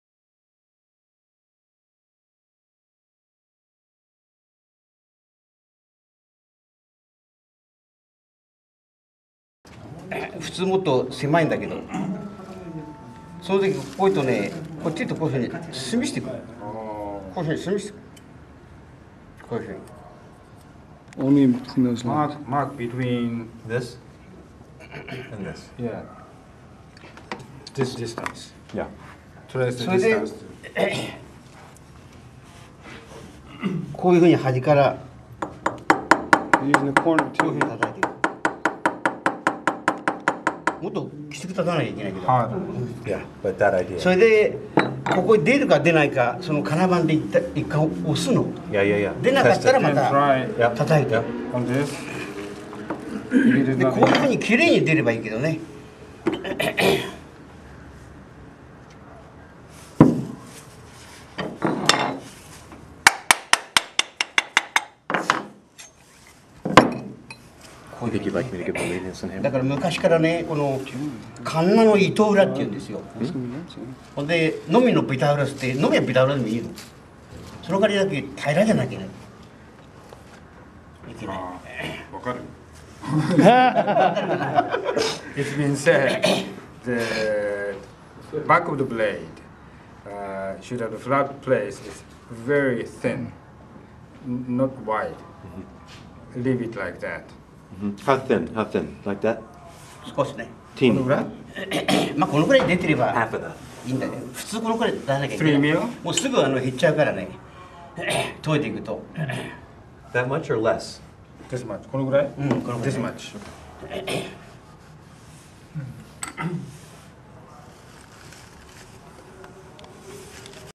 Ito San Master Japanese Carpenter Tapping Out a Kana Blade
This video shows Ito san, the Master Japanese Carpenter, showing Garrett Hack how to tap out a blade for a Kana, you have to do this when the flat on the back of the blade is worn out. Garrett Hack from Vermont was here in Tokyo for a few days, we were able to arrange a class/demo with Garrett showing his great skill and knowledge of woodworking, especially with hand planes. This was during one of the breaks in the class.